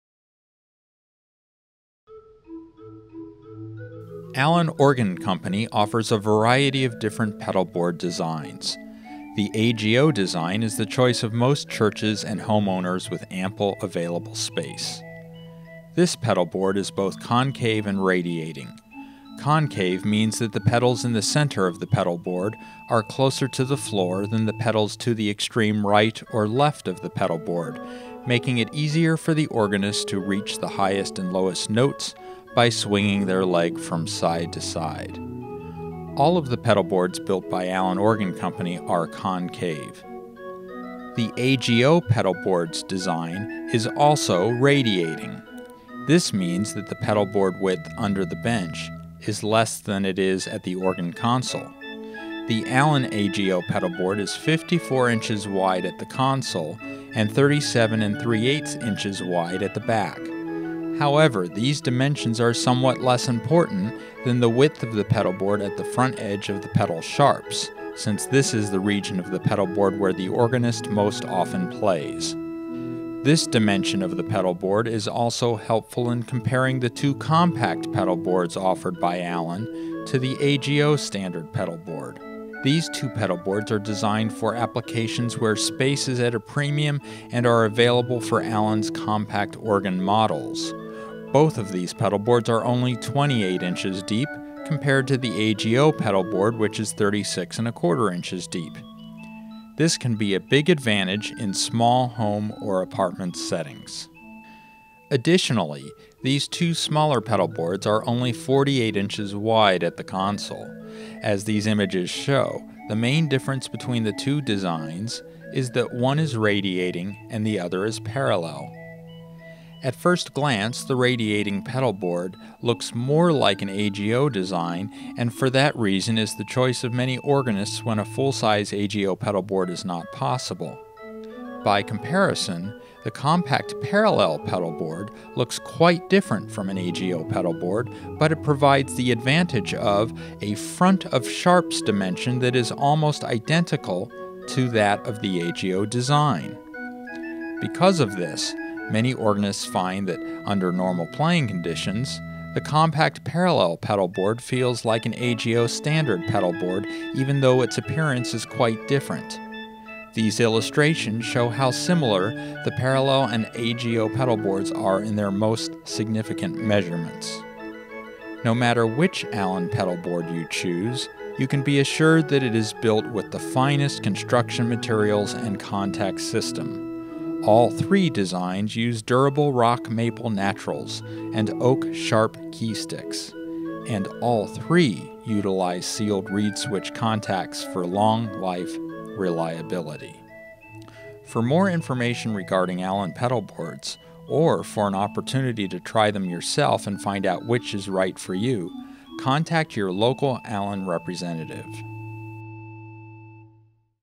Allen Organ Pedalboard Comparison: AGO and Compact
A must see video when considering which Compact Pedalboard is right for you! Allen Organ takes an in-depth look comparing its AGO Standard pedalboard to both the Allen Compact Radiating and Compact Parallel pedalboards found on Allen's Compact Organ models. Dimension likeness and playability area dimensions are discussed.